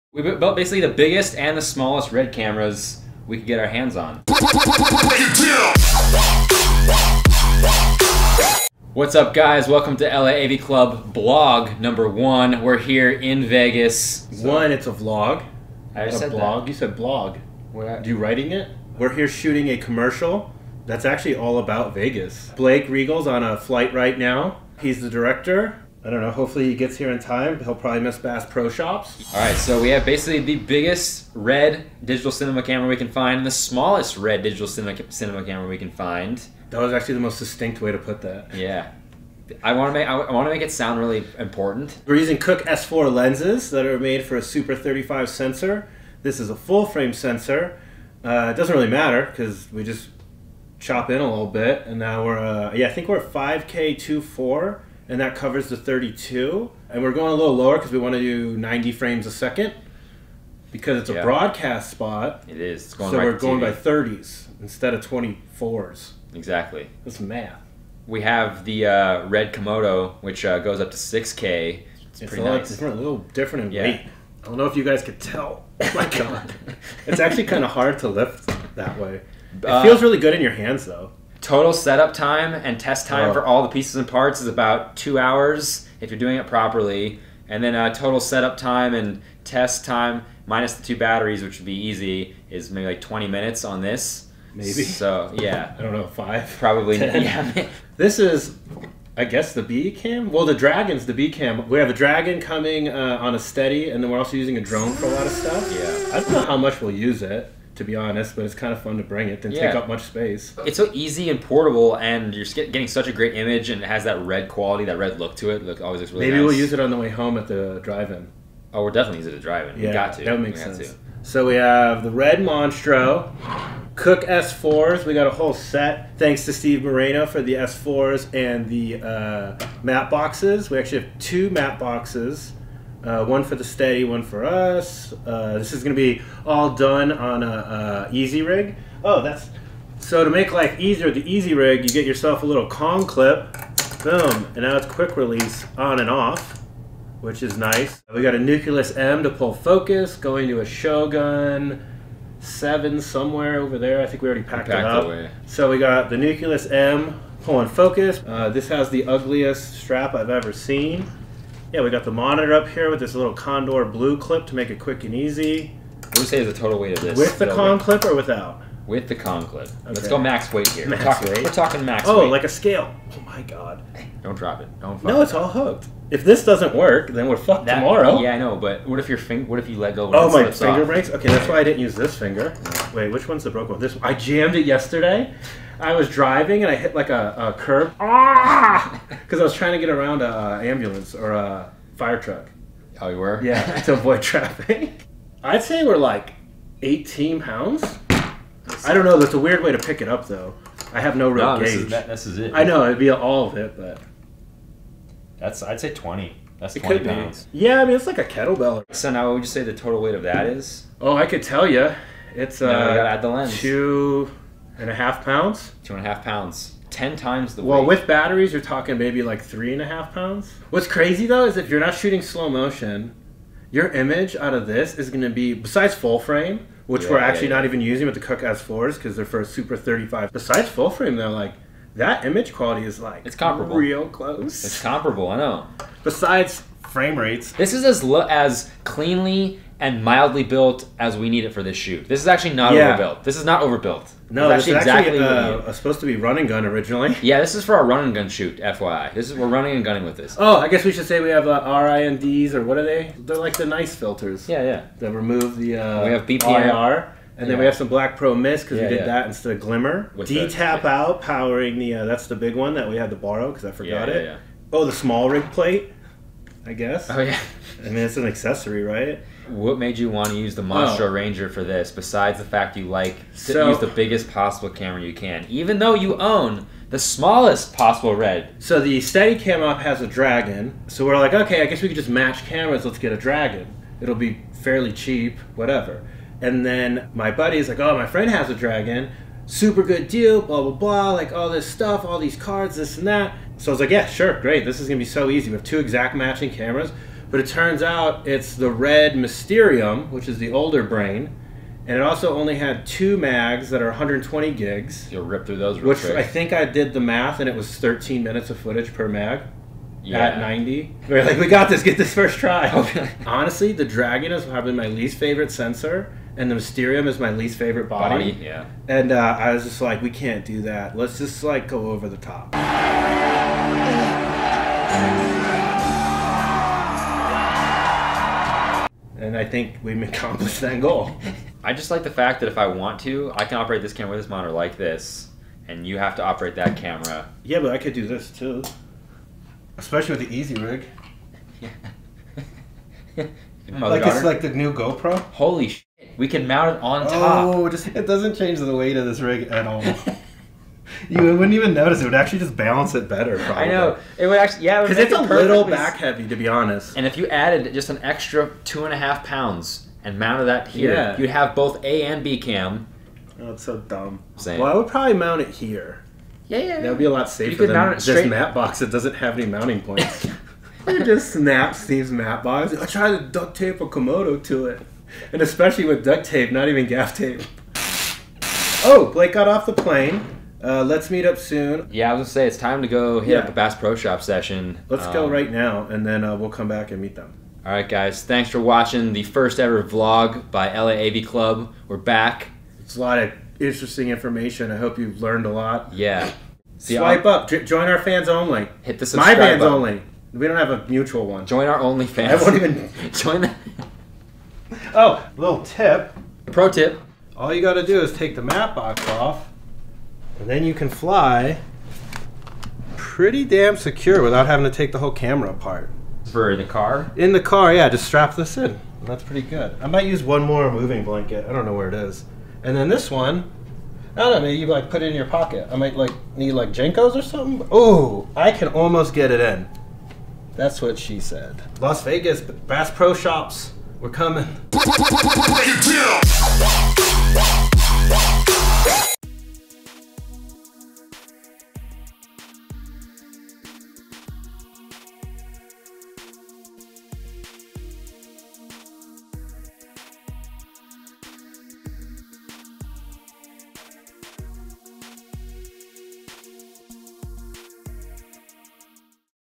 Red Komodo and Red Monstro Ranger: Biggest And Smallest Red Camera Prep: VLOG #1
LA AV VLOG #1

The Camera Jabronis discuss setting up and prepping the new Red Komodo and Red Monstro for a broadcast commercial shoot in Las Vegas. The Monstro Ranger is Reds biggest cinema camera and creates the nicest image while the Komodo is Red's newest camera thats creates an amazing image at 20% of the size.